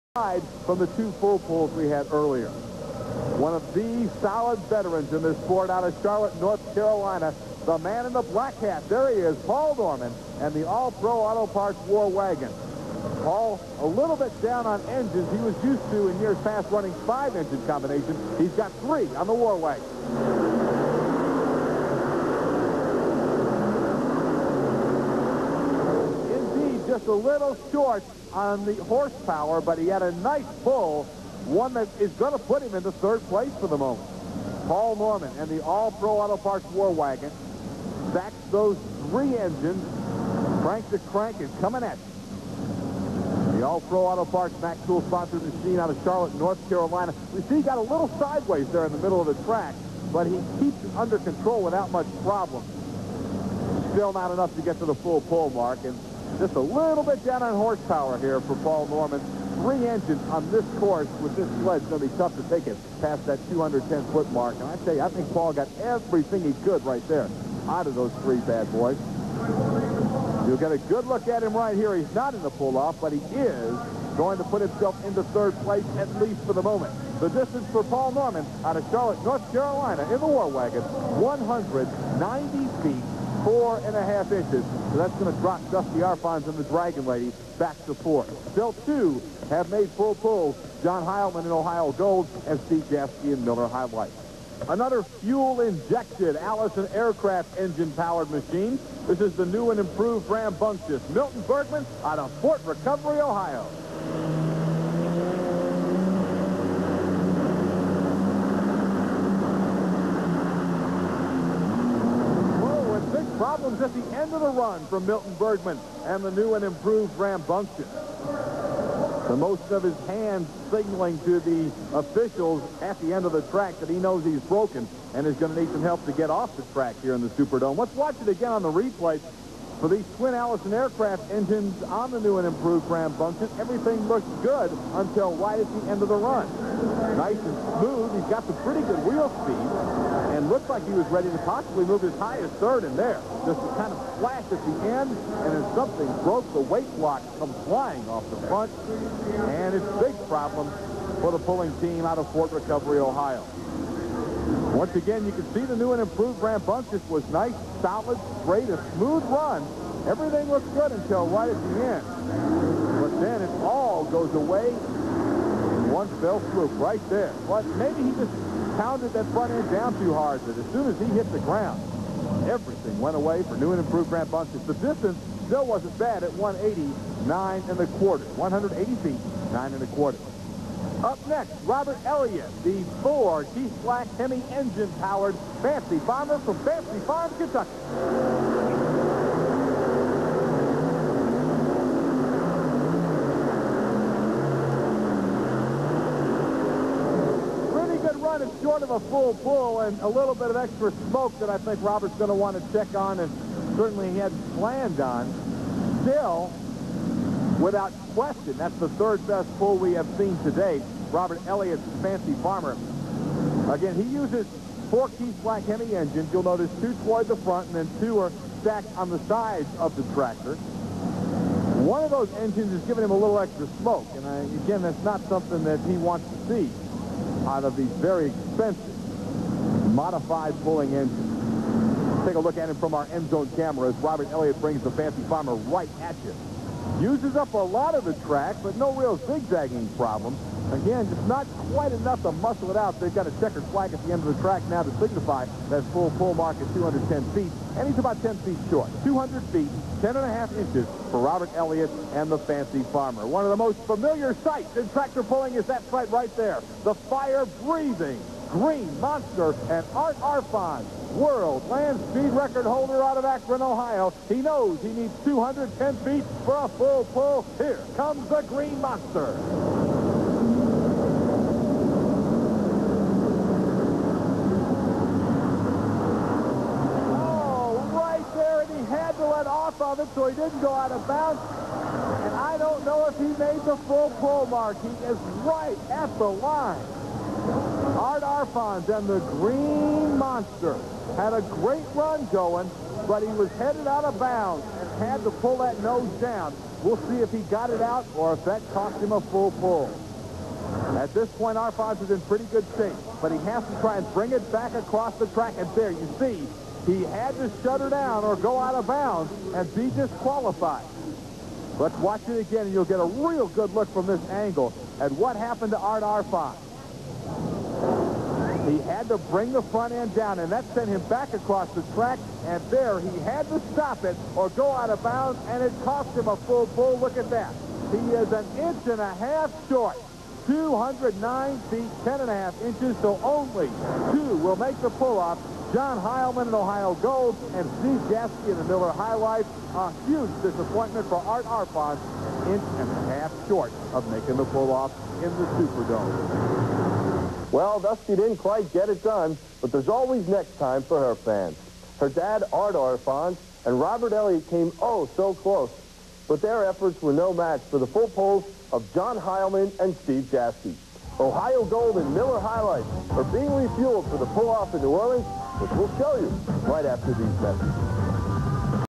USHRA Oakland 1992 Race Two Part Two
USHRA monster truck action from the Oakland Coliseum in Oakland, California on Super Trax. Also included is Modified Tractor Pulling from the Superdome in New Orleans, Louisiana.

Hosted By: Tom Baldrick
Commentary By: Scott Douglass